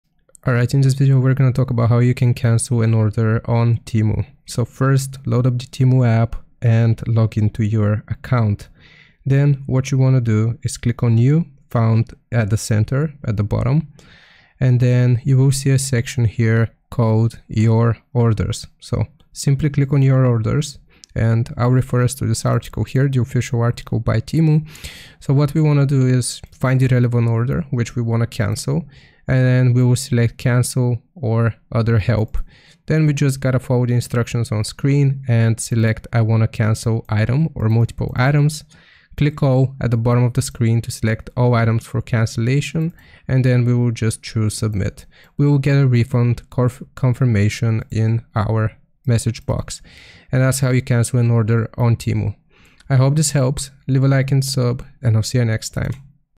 How To Cancel Order On Temu 2025
In this video I show you how to cancel an order on Temu in 2025. If you want to learn how to cancel Temu orders then this is the tutorial for you.